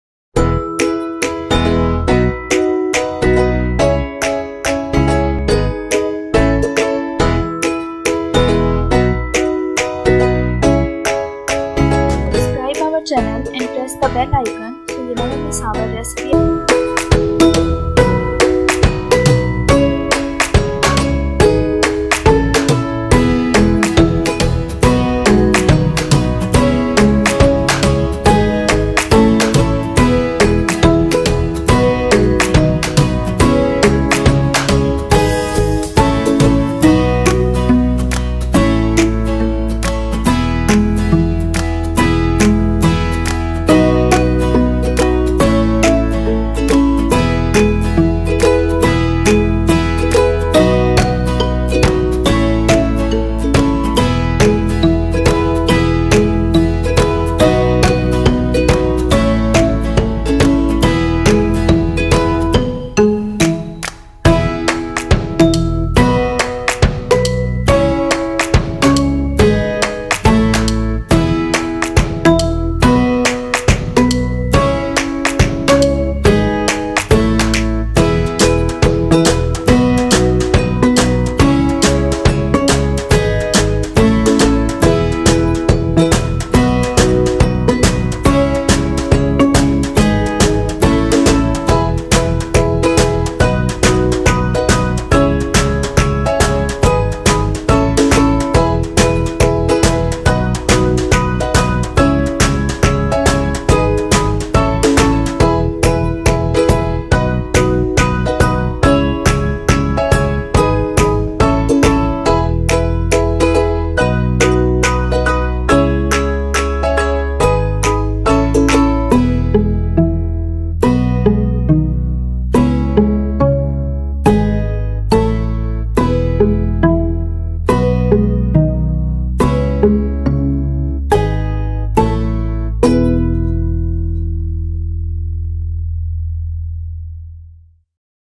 Sooji ka Halwa Recipe by Cooking with Fatima
Hi, in this video I am going to show you How to make "Sooji ka Halwa" at home.